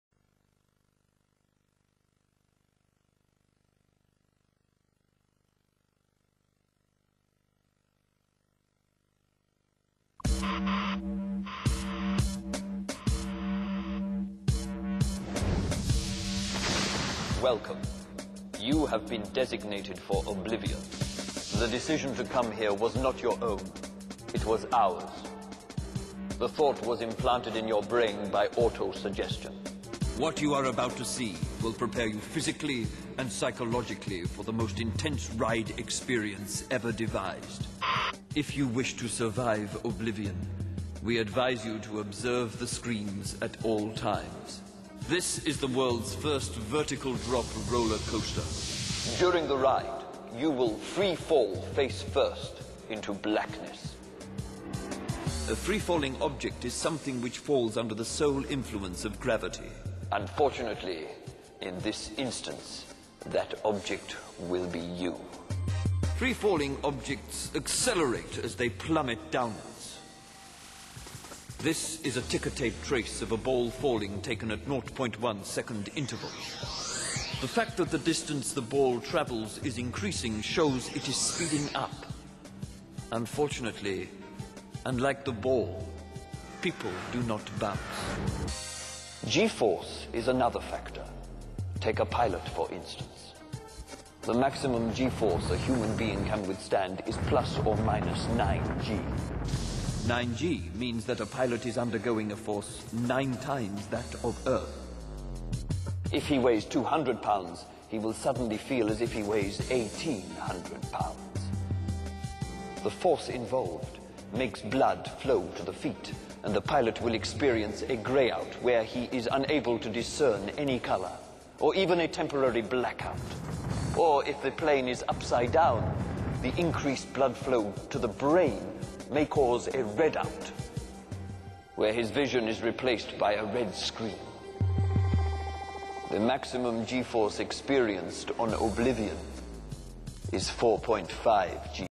Alton Towers - Oblivion The Lord Of Darkness (Rennie Krupinski)
Rennie Krupinski who starred as the Lord of Darkness as a themed character on Oblivion giving the ride a storyline, effects and drama.
Visit Rennies website here as he has starred in many shows